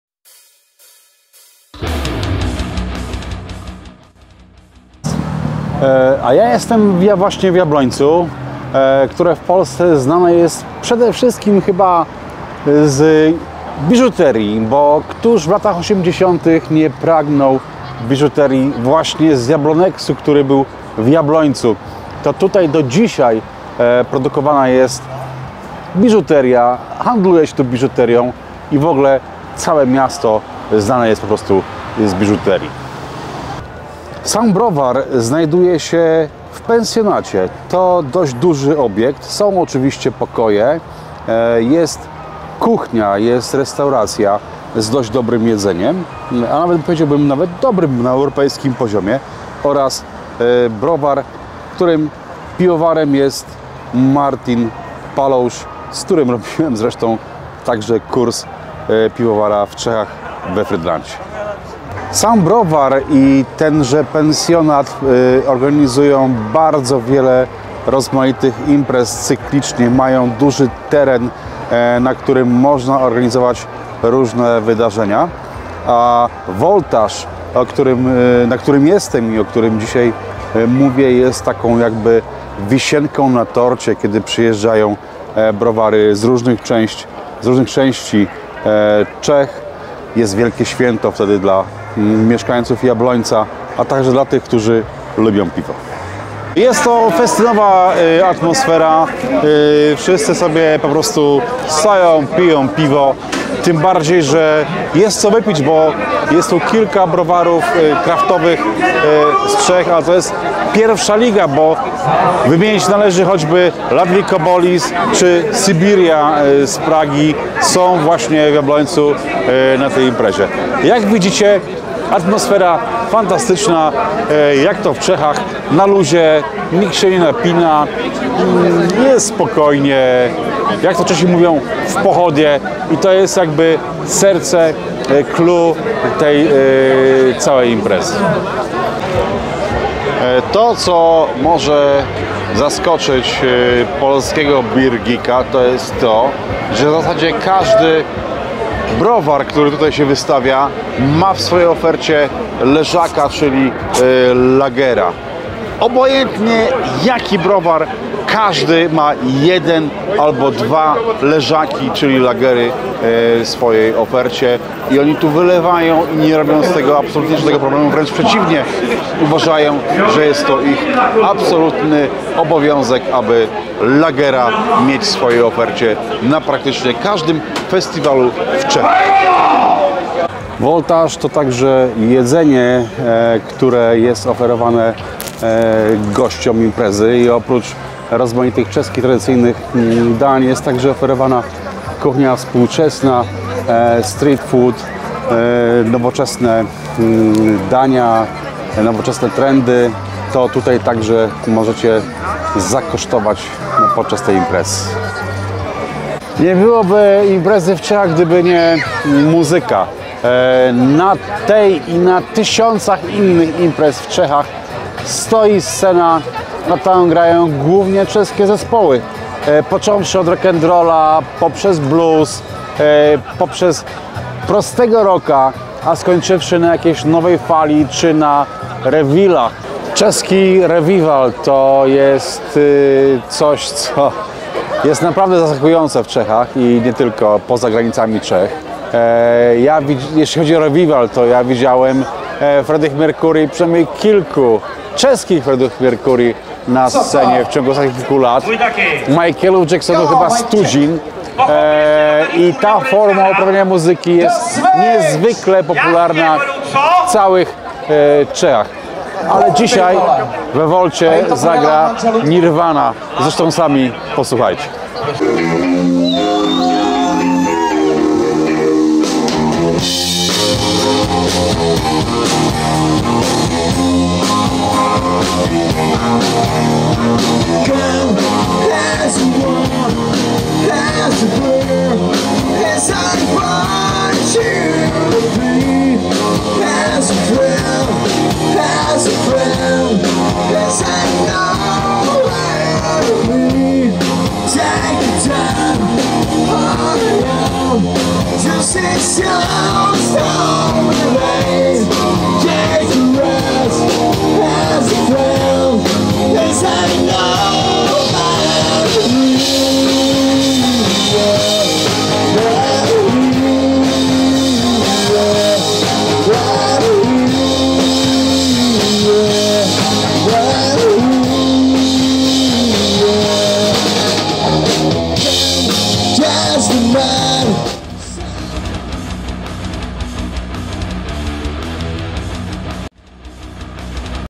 Voltáž 2022 Jablonec nad Nisou - jak tam było?
Amatorska relacja z minifestiwalu piwa Voltáž 2022 w Browarze Volt w Jabloncu nad Nisou (CZ). Przegląd miejsca wokół browaru, ciekawostki, migawki z koncertów.